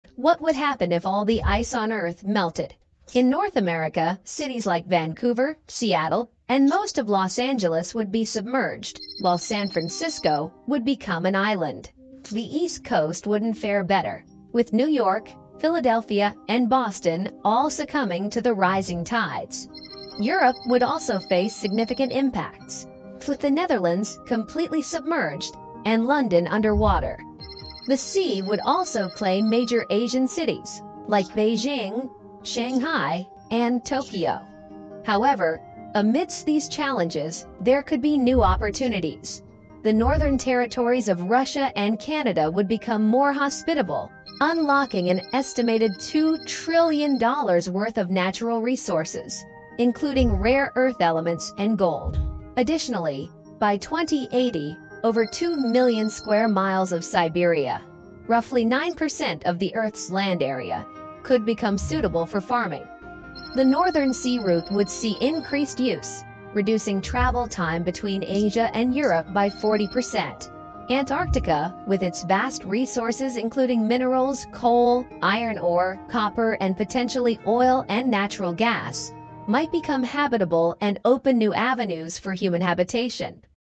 What If All the Ice On Earth Melts Global Flood🌎
🌍 What if all the Earth’s ice melted? 🧊 Sea levels would rise by 66 meters, flooding coastlines and submerging entire cities. 🗽 New York would vanish, 🌴 Florida would almost disappear, and 🌉 San Francisco would become an island. In Europe, 🇳🇱 the Netherlands and 🇩🇰 Denmark would face full submersion, while 🇬🇧 the UK would lose coastal areas. 🛶 Venice gone, and parts of 🌆 Barcelona and 🏛 Rome flooded. Across Asia, 🗼 Tokyo, 🌇 Hong Kong, and 🕌 Bangkok would be partially underwater, and 🇧🇩 Bangladesh nearly wiped off the map. Africa and Australia would see dramatic coastline changes, with major cities erased. But that’s just the start. Without ice to reflect sunlight, 🌡️ the Earth would heat up fast — triggering 🔥 wildfires, 🌵 droughts, and 🌊 floods. The 🌊 Gulf Stream could collapse, freezing Europe and drying Africa. Glaciers would melt, creating a global 🌐 freshwater crisis. This isn’t a sci-fi movie — it’s a warning from the planet. 🌎